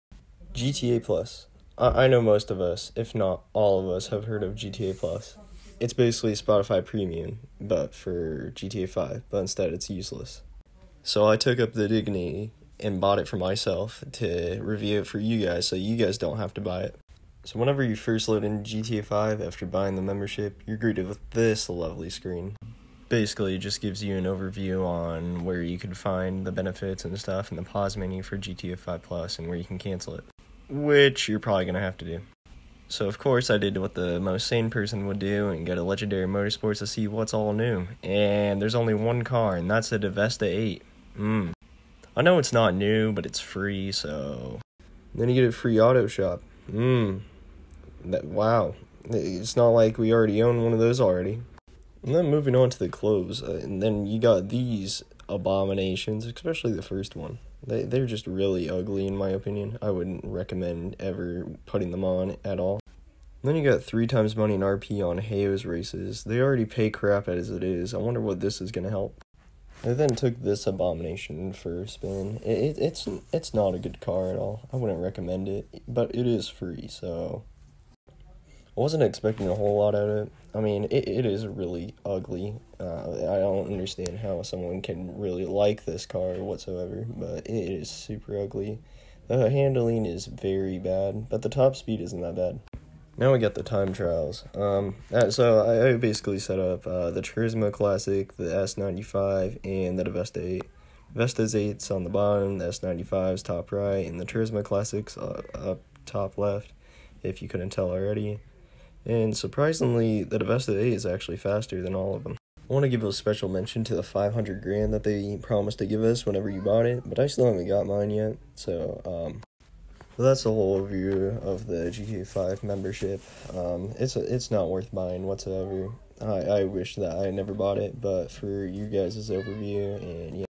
Gta+ summary and overview + my personal thoughts
If you don't want to watch the video all the way it's not good whatsoever I wouldn't recommend buying it.